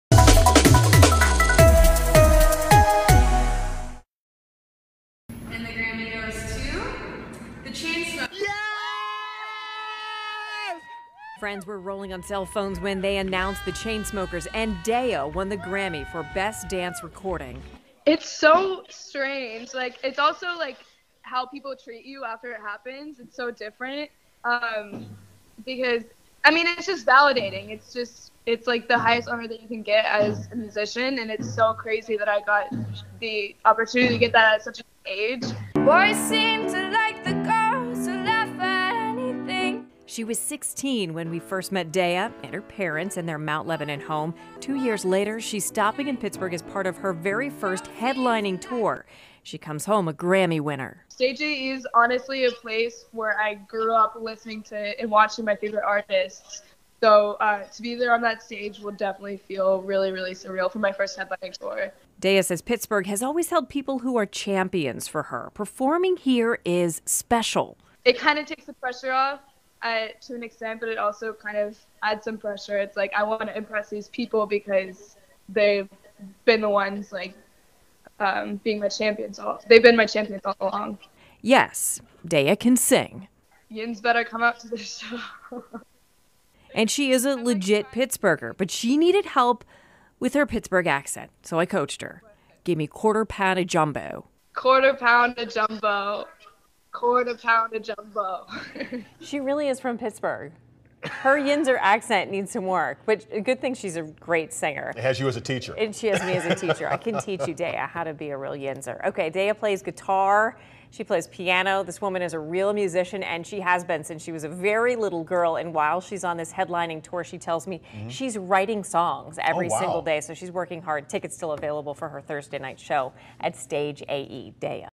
One-on-one with Daya
Shannon Perrine Facetimed with local Grammy Winner Daya.